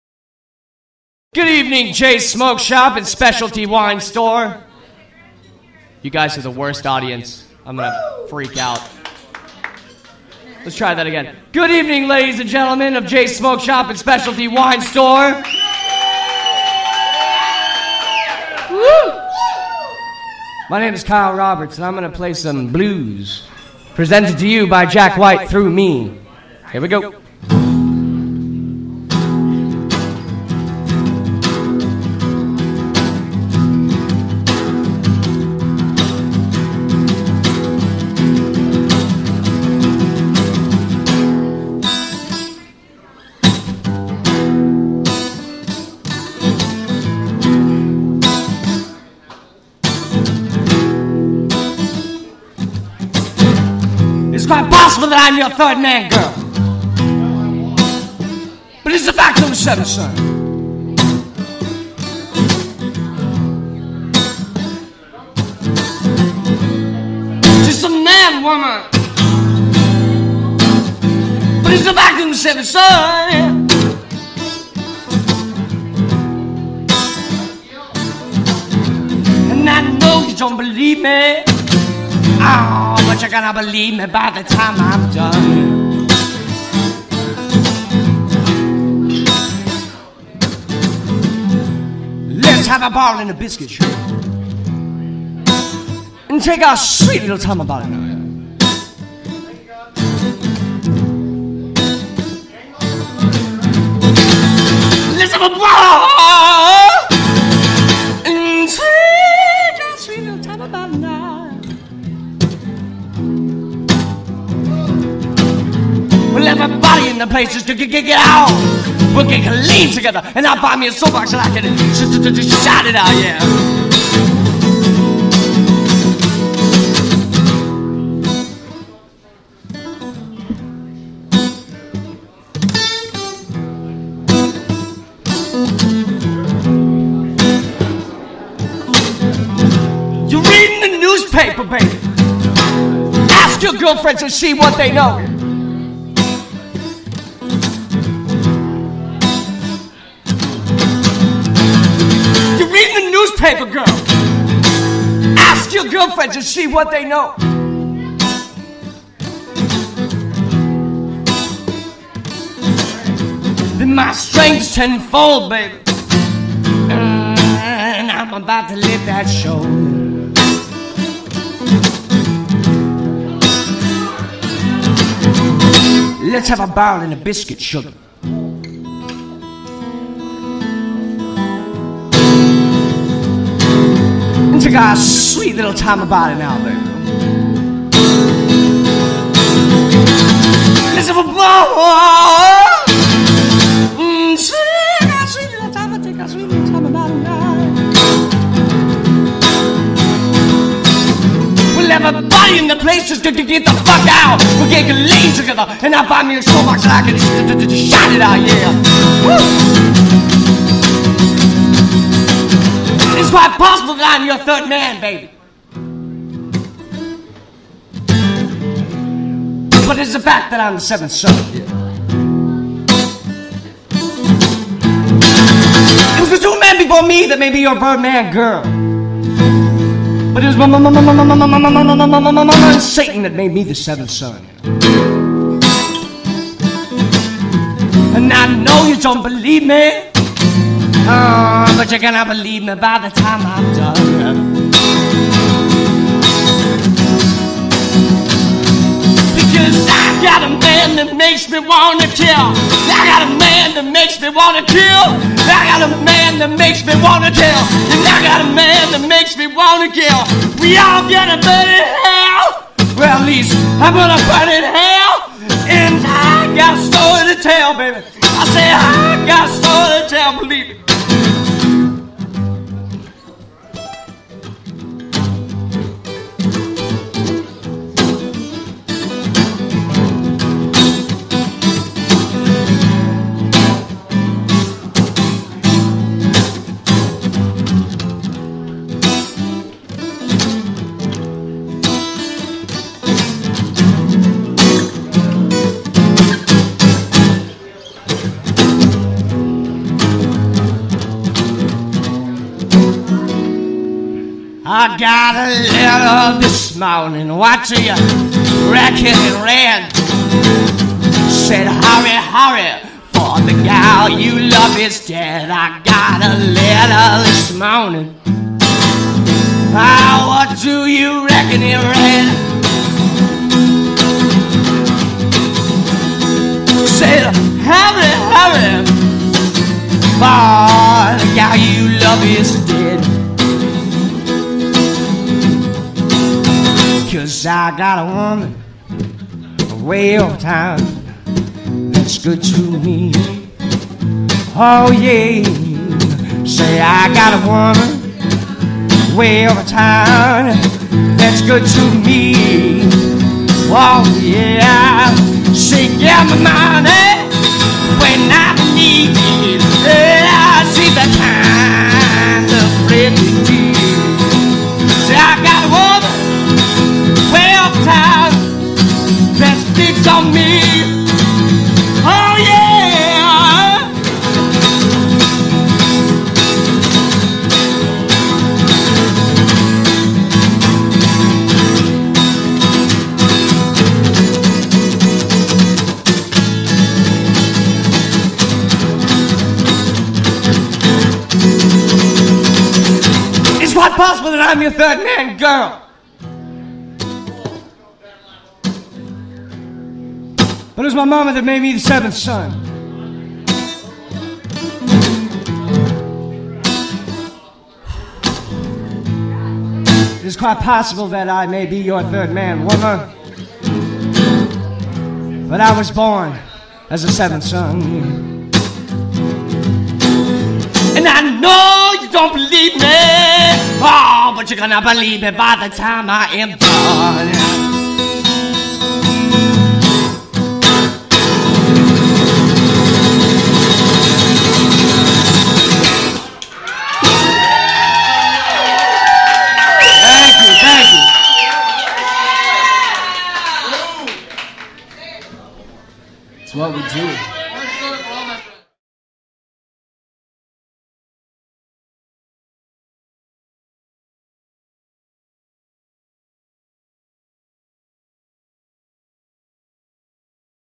Kyle Roberts @ J's Smoke Shop
Kyle Roberts performing at J's Smoke Shop in Lake Havasu City, Arizona